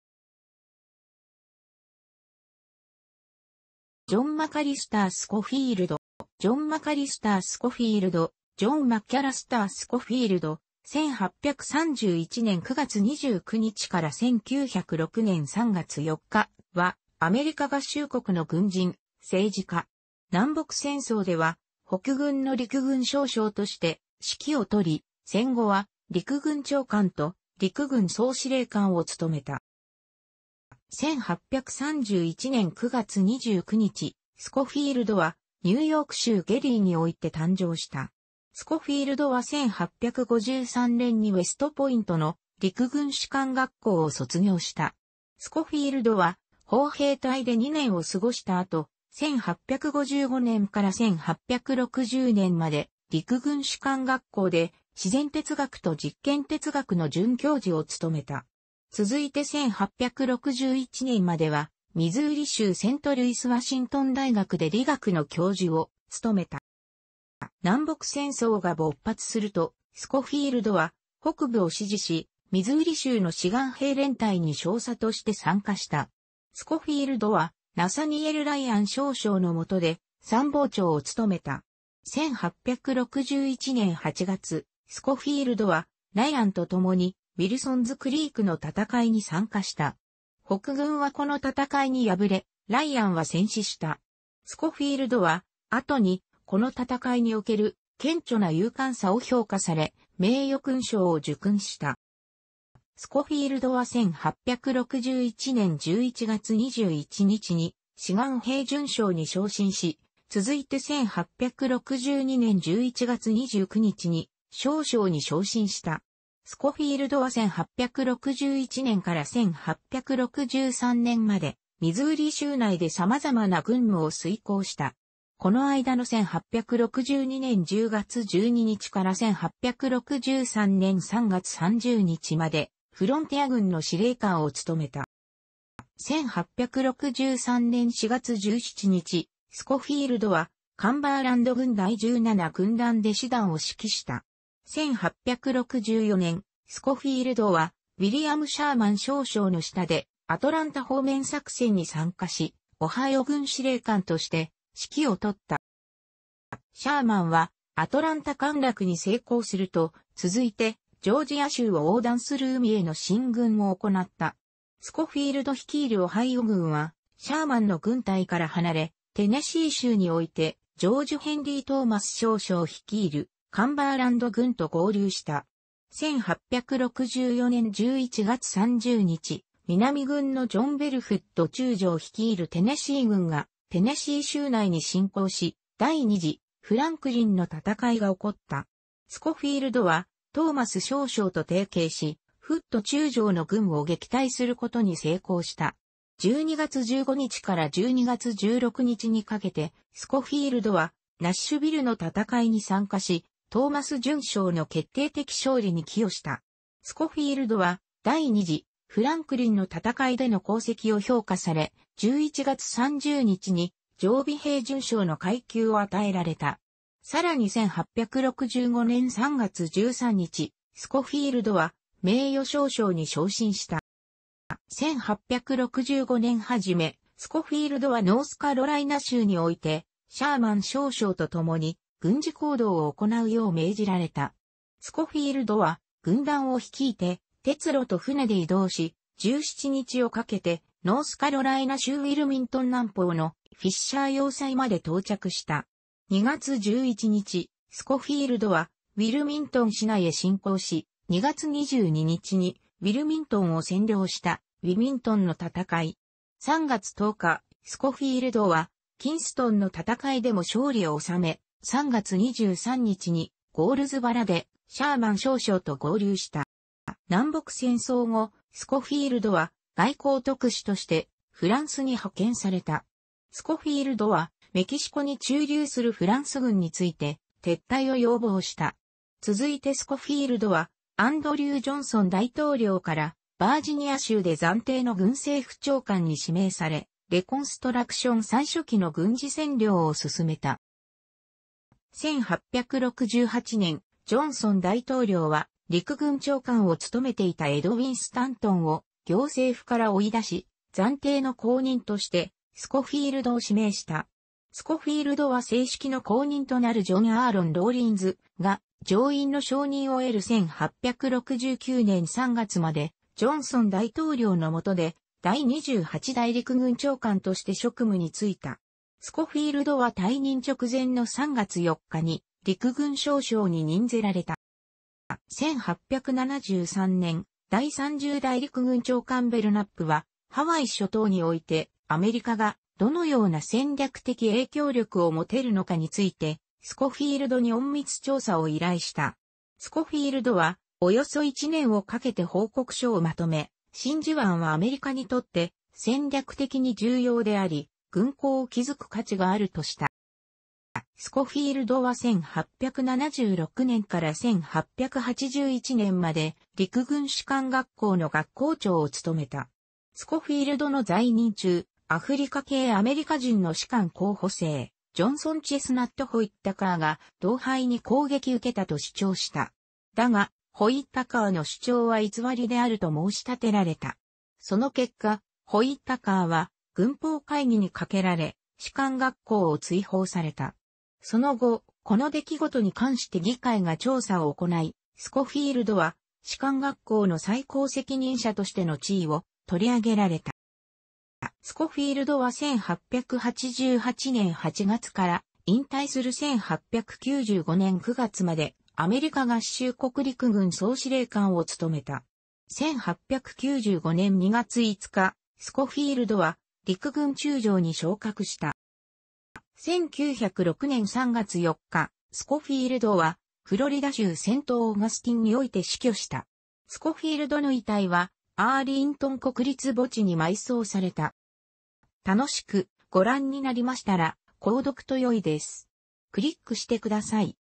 ジョン・マカリスター・スコフィールド
セントルイス・ワシントン大学の教員

ジョン・マカリスター・スコフィールド（John McAllister Schofield, 1831年9月29日 - 1906年3月4日）は、アメリカ合衆国の軍人、政治家。南北戦争では北軍の陸軍少将として指揮を執り、戦後は陸軍長官と陸軍総司令官を務めた。

1831年9月29日、スコフィールドはニューヨーク州ゲリーにおいて誕生した。スコフィールドは1853年にウェストポイントの陸軍士官学校を卒業した。スコフィールドは砲兵隊で2年を過ごした後、1855年から1860年まで陸軍士官学校で自然哲学と実験哲学の准教授を務めた。続いて1861年までは、ミズーリ州セントルイス・ワシントン大学で物理学の教授を務めた。

南北戦争が勃発すると、スコフィールドは北部を支持し、ミズーリ州の志願兵連隊に少佐として参加した。スコフィールドはナサニエル・ライアン少将の下で参謀長を務めた。1861年8月、スコフィールドはライアンとともにウィルソンズ・クリークの戦いに参加した。北軍はこの戦いに敗れ、ライアンは戦死した。スコフィールドは後に、この戦いにおける「顕著な勇敢さ」を評価され、名誉勲章を受勲した。

スコフィールドは1861年11月21日に志願兵准将に昇進し、続いて1862年11月29日に少将に昇進した。スコフィールドは1861年から1863年までミズーリ州内でさまざまな軍務を遂行した。この間の1862年10月12日から1863年3月30日まで、フロンティア軍の司令官を務めた。

1863年4月17日、スコフィールドはカンバーランド軍第17軍団で師団を指揮した。1864年、スコフィールドはウィリアム・シャーマン少将の下でアトランタ方面作戦に参加し、オハイオ軍司令官として指揮を執った。

シャーマンはアトランタ陥落に成功すると、続いてジョージア州を横断する海への進軍を行った。スコフィールド率いるオハイオ軍はシャーマンの軍隊から離れ、テネシー州においてジョージ・ヘンリー・トーマス少将率いるカンバーランド軍と合流した。1864年11月30日、南軍のジョン・ベル・フッド中将率いるテネシー軍がテネシー州内に侵攻し、第二次フランクリンの戦いが起こった。スコフィールドはトーマス少将と提携し、フッド中将の軍を撃退することに成功した。12月15日から12月16日にかけて、スコフィールドはナッシュヴィルの戦いに参加し、トーマス准将の決定的勝利に寄与した。スコフィールドは第二次フランクリンの戦いでの功績を評価され、11月30日に常備兵准将の階級を与えられた。さらに1865年3月13日、スコフィールドは名誉少将に昇進した。

1865年初め、スコフィールドはノースカロライナ州において、シャーマン少将とともに軍事行動を行うよう命じられた。スコフィールドは軍団を率いて鉄路と船で移動し、17日をかけてノースカロライナ州ウィルミントン南方のフィッシャー要塞まで到着した。2月11日、スコフィールドはウィルミントン市内へ侵攻し、2月22日にウィルミントンを占領した（ウィルミントンの戦い）。3月10日、スコフィールドはキンストンの戦いでも勝利を収め、3月23日にゴールズバラでシャーマン少将と合流した。

南北戦争後、スコフィールドは外交特使としてフランスに派遣された。スコフィールドはメキシコに駐留するフランス軍について、撤退を要望した。続いてスコフィールドはアンドリュー・ジョンソン大統領からバージニア州で暫定の軍政府長官に指名され、レコンストラクション最初期の軍事占領を進めた。

1868年、ジョンソン大統領は陸軍長官を務めていたエドウィン・スタントンを行政府から追い出し、暫定の後任としてスコフィールドを指名した。スコフィールドは正式の後任となるジョン・アーロン・ローリンズが上院の承認を得る1869年3月まで、ジョンソン大統領の下で第28代陸軍長官として職務に就いた。スコフィールドは退任直前の3月4日に、陸軍少将に任ぜられた。

1873年、第30代陸軍長官ベルナップは、ハワイ諸島においてアメリカがどのような戦略的影響力を持てるのかについて、スコフィールドに隠密調査を依頼した。スコフィールドはおよそ1年をかけて報告書をまとめ、真珠湾はアメリカにとって戦略的に重要であり、軍港を築く価値があるとした。